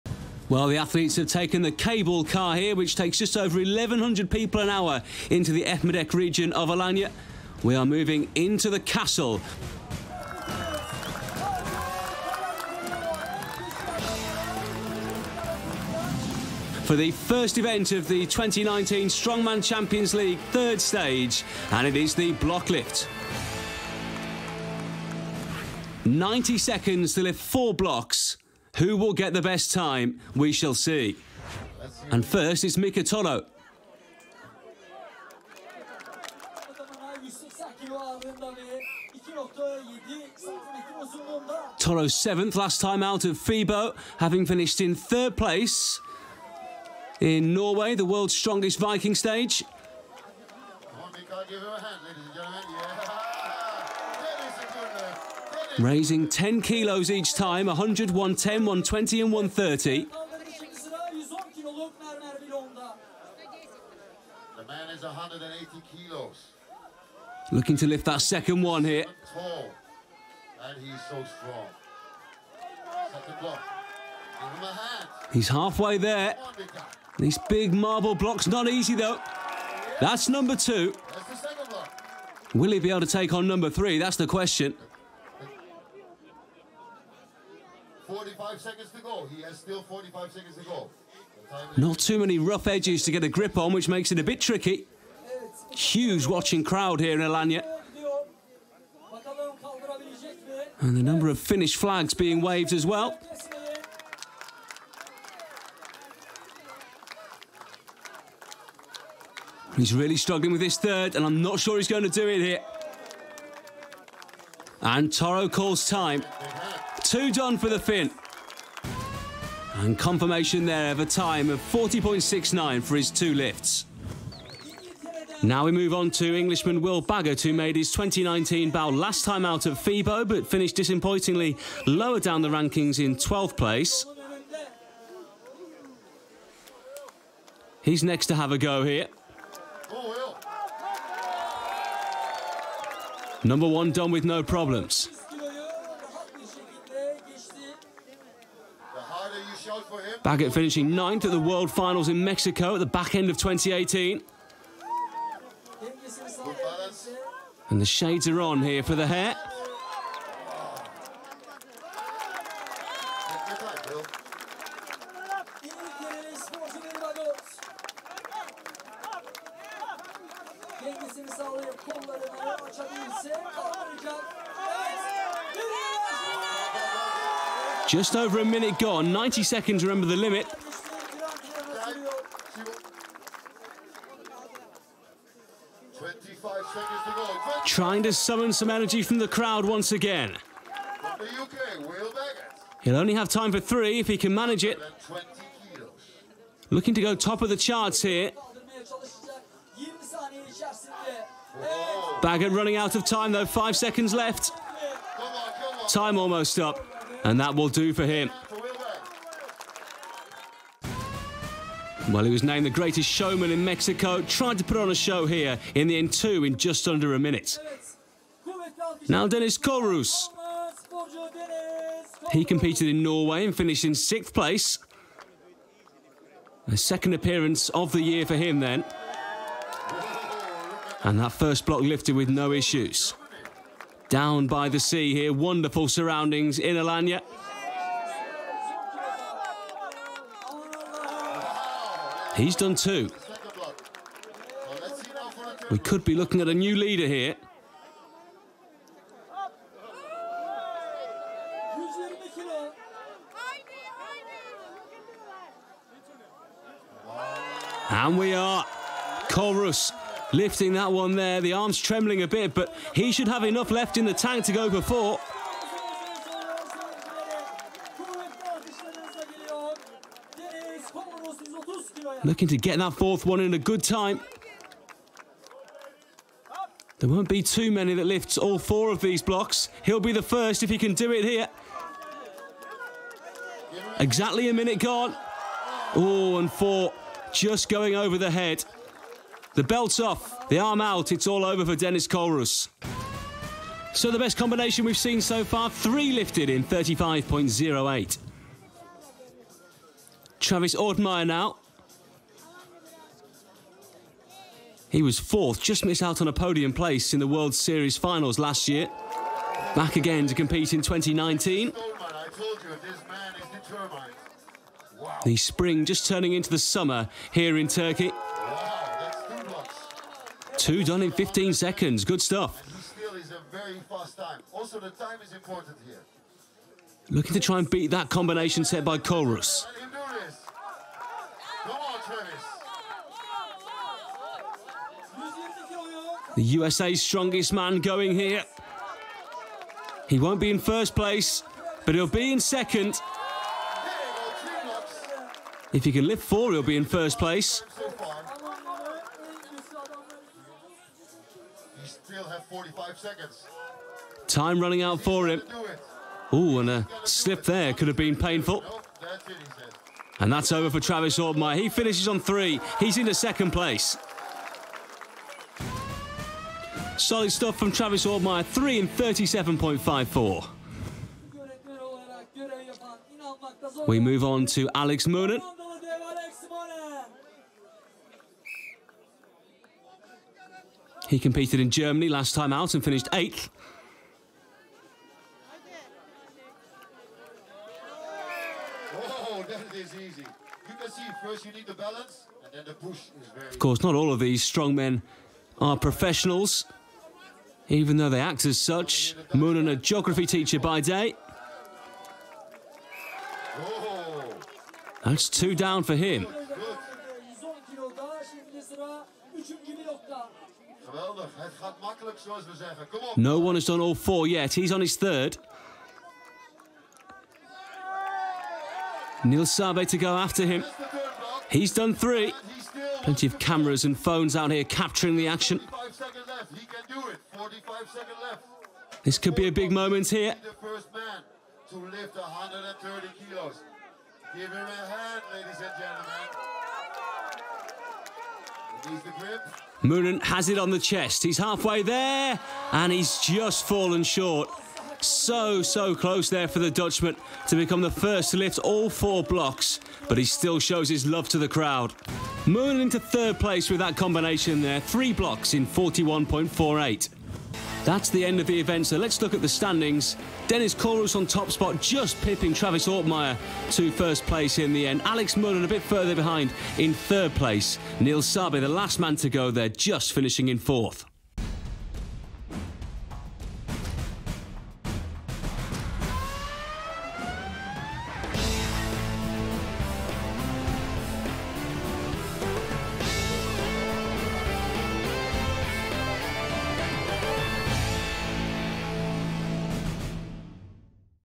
Dennis Kohlruss Beats Travis Ortmayer In 130KG Block Lift | Strongman Champions League
Daniel Kohlruss lifts his way to victory in the block lift event, lifting 100kg to 130kg in quickest time, beating the likes of Alex Moonen, Travis Ortmayer and Will Baggot in the SCL Turkey event

From Strongman Champions League: Turkey 2019